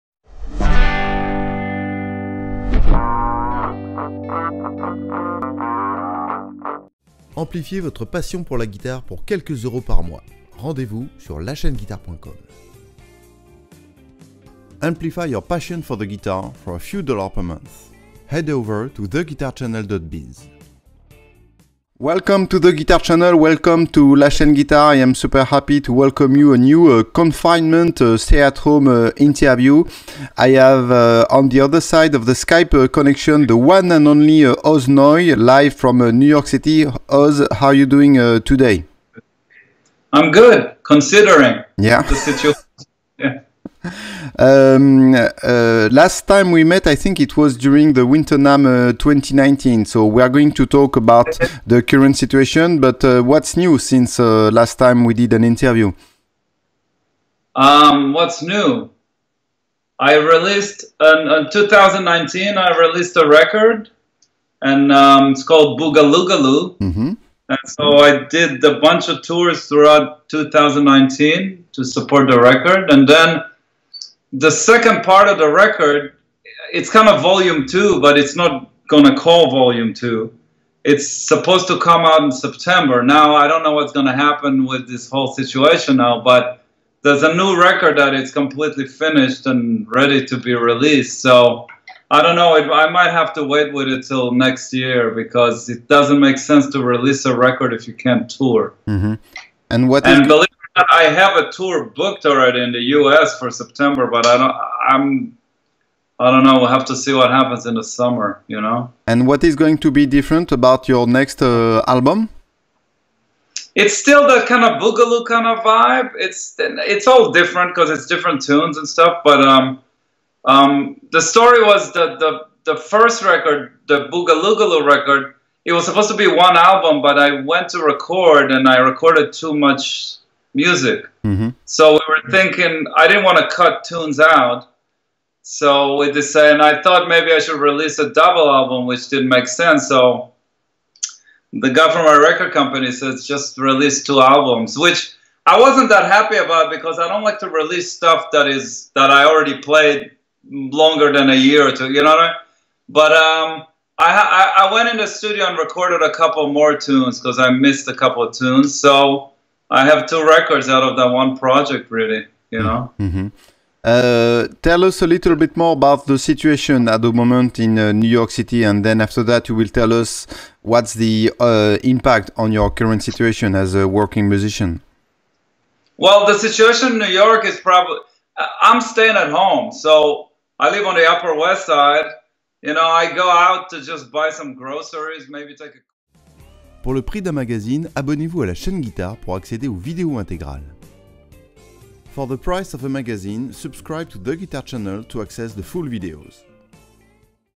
Oz Noy live from New York City - Coronavirus Stay-at-Home interview 15/04/20
The Israel born and US based guitar player Oz Noy was my guest for this Stay-at-Home interview. He talks about his current projects and how he is going through the weird times we are living at the moment.

Le guitariste né en Israël et basé aux Etats-Unis Oz Noy était l'invité de cette interview confinement. Il parle de ses projets en cours, de son futur album et de la manière qu'il traverse cette période étrange.

Envie de regarder plus de vidéos guitare ? Bénéficiez de 30 jours gratuits sur La Chaîne Guitare :